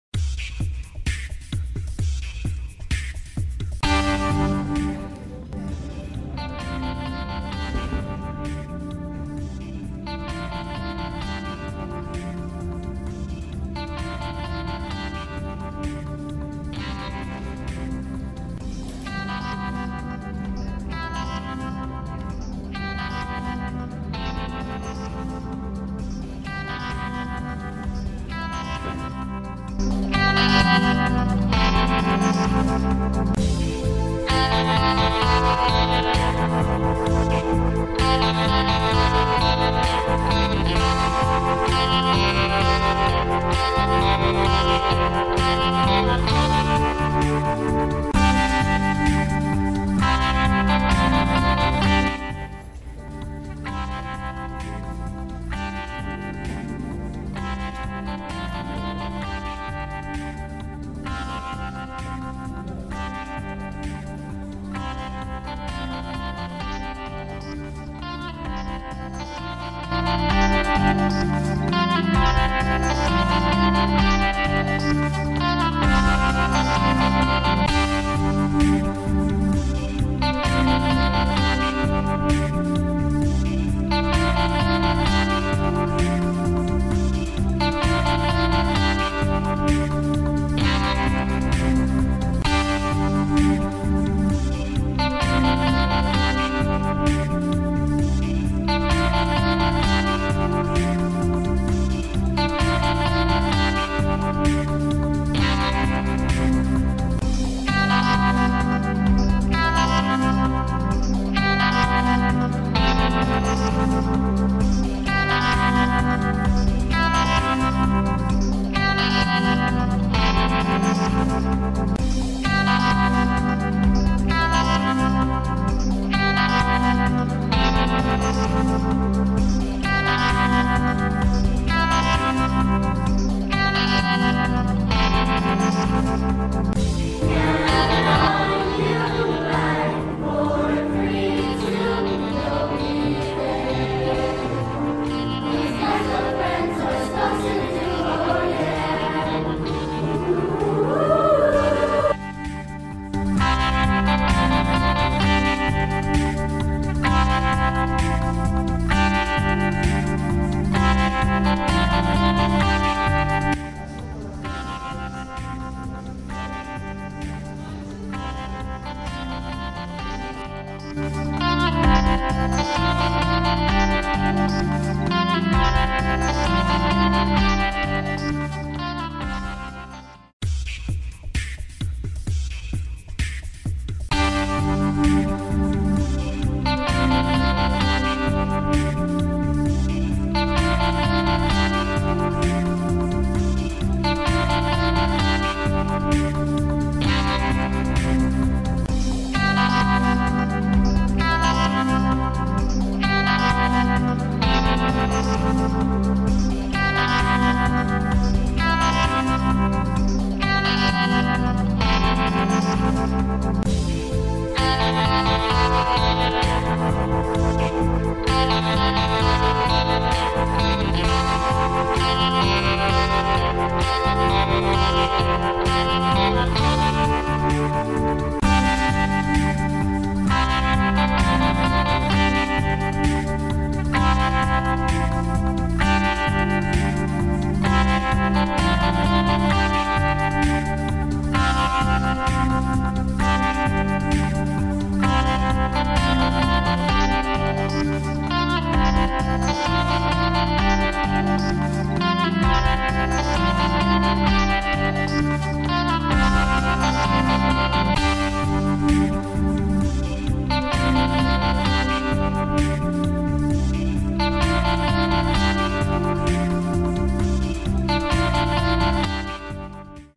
Manchester Youth Arts Festival 2019
Artwork from all of the district's schools was on display at the Manchester Youth Arts Festival on May 16, 2019 at Manchester Township High School.

This video includes photos of the art displays.